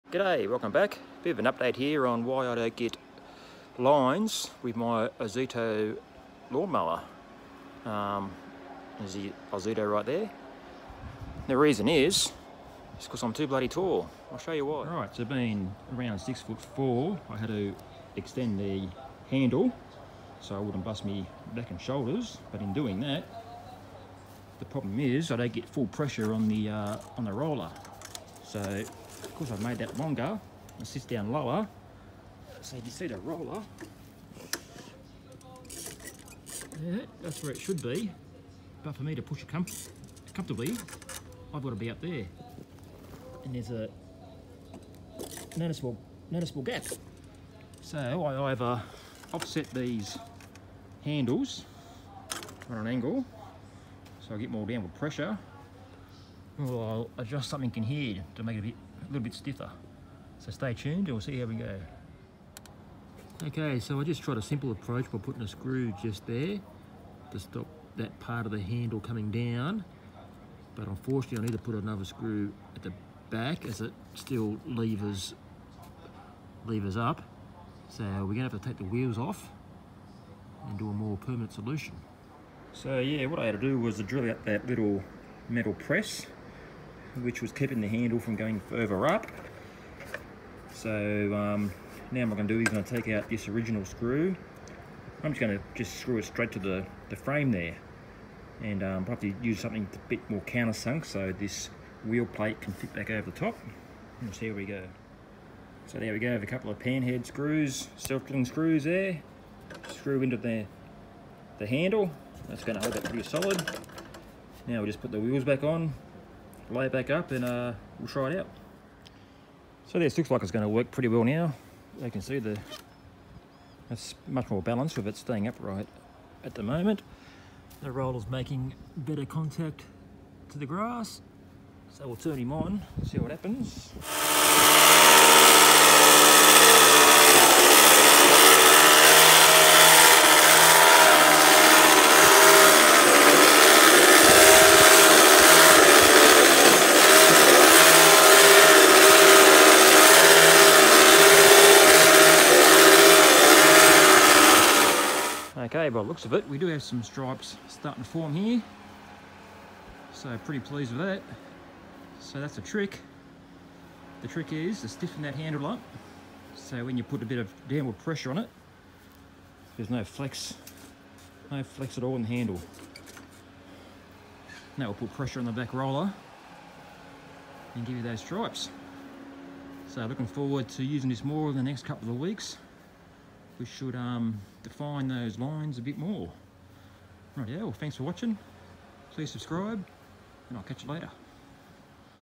Striping with the Ozito Cylinder Mower
With a modification to the handle my Ozito does do strips.